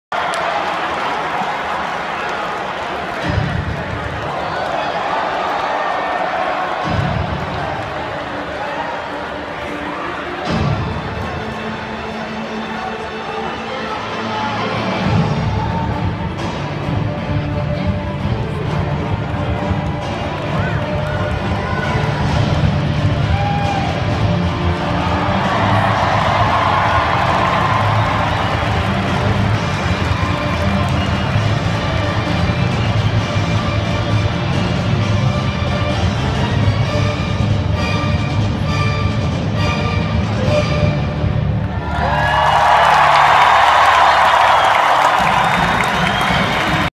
Tennessee Tech Men's Basketball Intro
Brand new 3D projection pregame introduction for the basketball team.